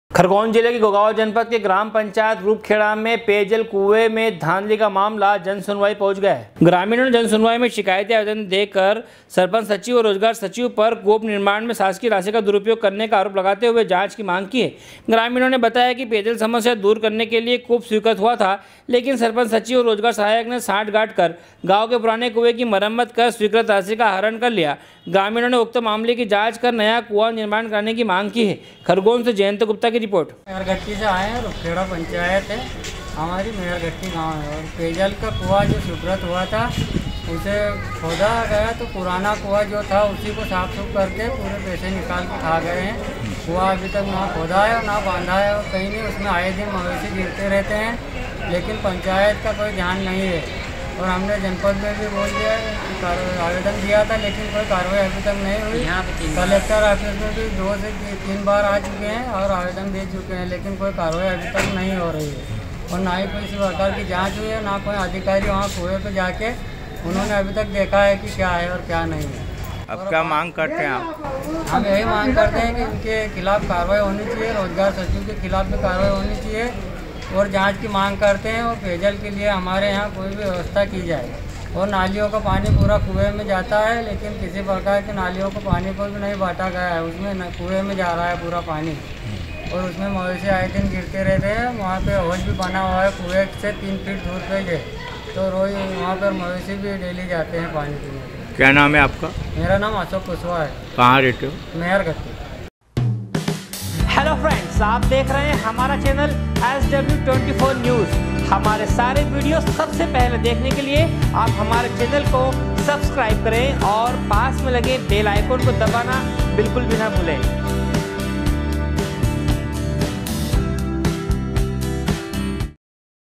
SW 24 News Madhyapradesh
खरगोन जिले मे गोगावां जनपद के ग्राम पंचायत रूपखेड़ा में पेयजल कुए में धांधली का मामला जनसुनवाई पहुचा है। खरगोन से जयंत गुप्ता की रिपोर्ट